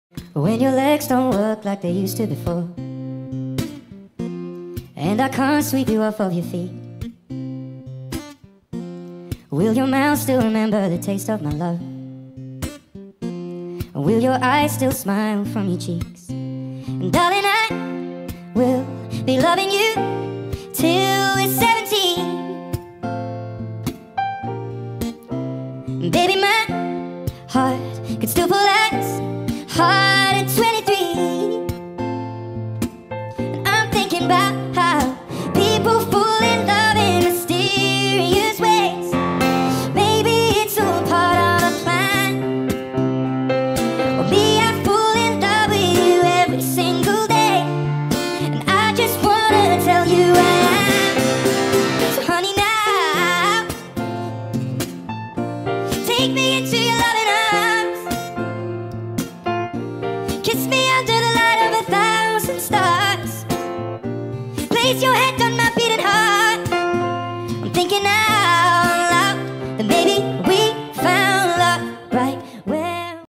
Sony Vegas Pro Effect Tutorial #3
Hey this effect was also request a ton so here it is :)
This effect is inspired by/kinda remake of @frieswith5sauce on vine   Hope this helps you guys!